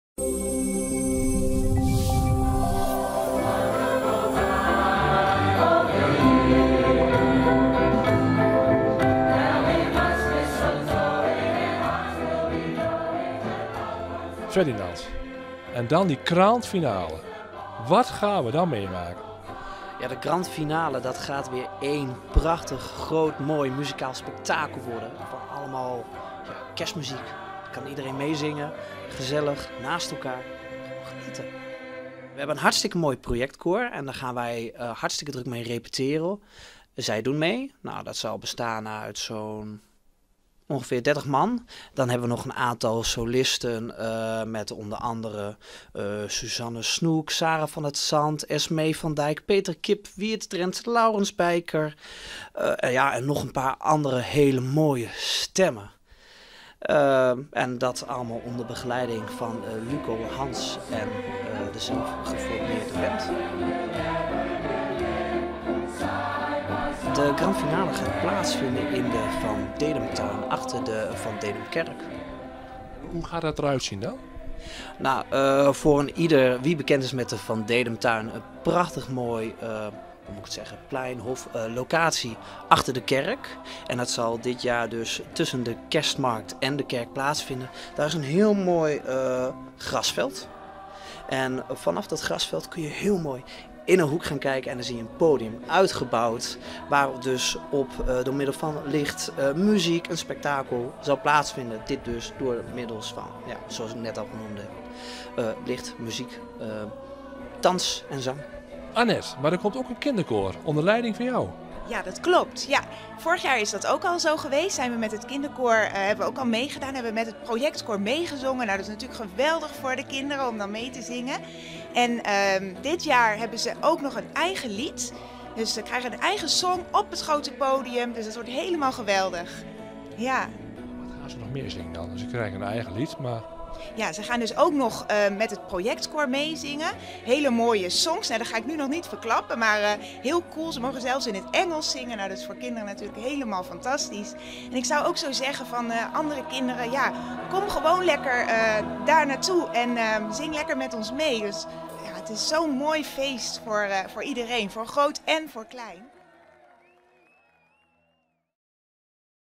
Dedemsvaart: info Grand Finale - Kerst an de Voart
Voor meer informatie over de Grand Finale die plaats vind op zaterdag 15 december tijdens Kerst an de Voart kijk ook op
* Te zien op televisie via wijZijn.tv en via Internet op alle apparaten op of via Instagram.